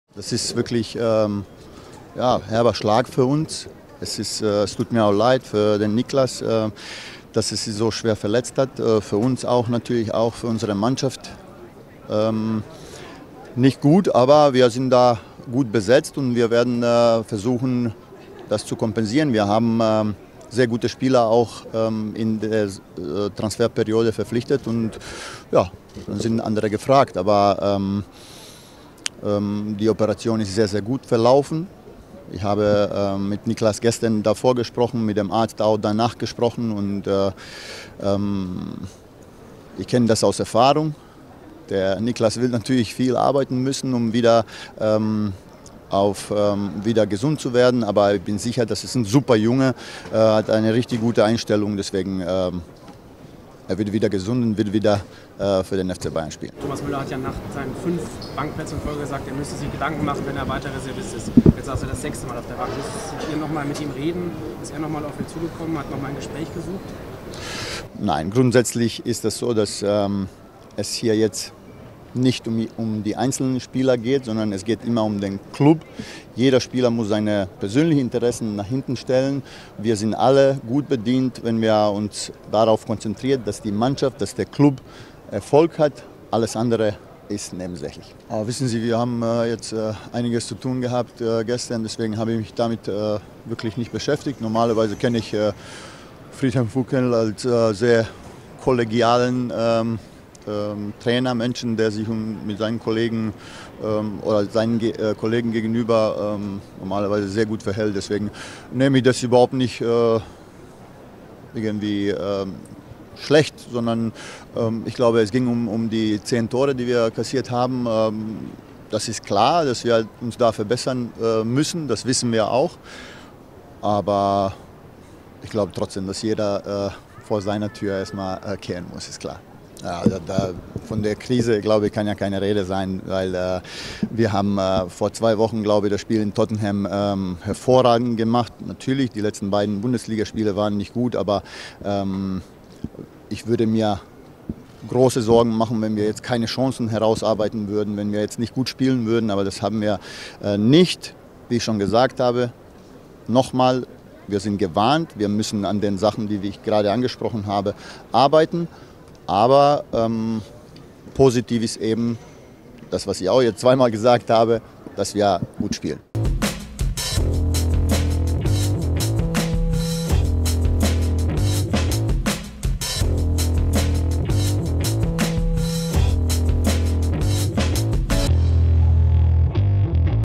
Hasan Salihamidzic ermahnt Thomas Müller | SPORT1
Bayerns Sportdirektor Hasan Salihamidzic äußert sich zu den Aussagen von Thomas Müller über dessen Reservistenrolle und ermahnt den Bayern-Star.